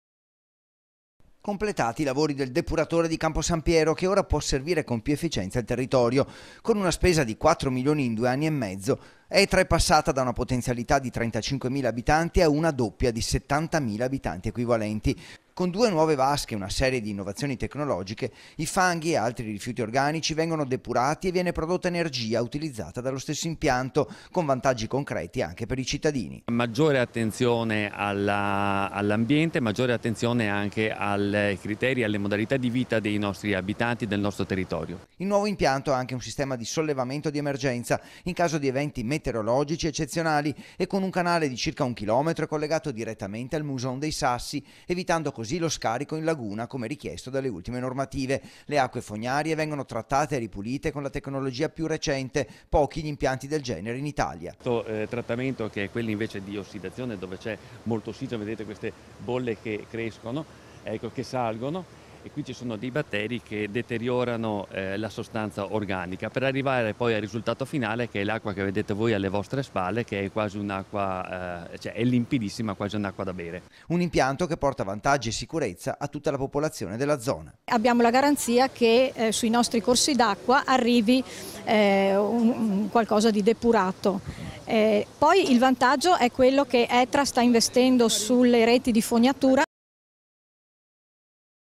TG (17/05/2018) - NUOVO DEPURATORE, POTENZIALITA’ RADDOPPIATA
TG (giovedì 17 maggio 2018) - Completati i lavori del depuratore di Camposampiero, che ora può servire con più efficienza il territorio. Con una spesa da parte di Etra di 4 milioni in 2 anni e mezzo, si è passati da una potenzialità di 35mila abitanti a una doppia di 70 mila abitanti equivalenti. Con due nuove vasche e una serie di innovazioni tecnologiche, i fanghi e altri rifiuti organici vengono depurati e viene prodotto energia utilizzata dallo stesso impianto. Con vantaggi concreti anche per i cittadini.Il nuovo impianto ha anche un sistema di sollevamento di emergenza in caso di eventi meteorologici eccezionali, e con un canale di circa 1 km è collegato direttamente al Muson dei Sassi, evitando così lo scarico in Laguna come richiesto dalle ultime normative. Le acque fognarie vengono trattate e ripulite con la tecnologia più recente, pochi gli impianti del genere in Italia.Un impianto che porta vantaggi e sicurezza a tutta la popolazione della zona.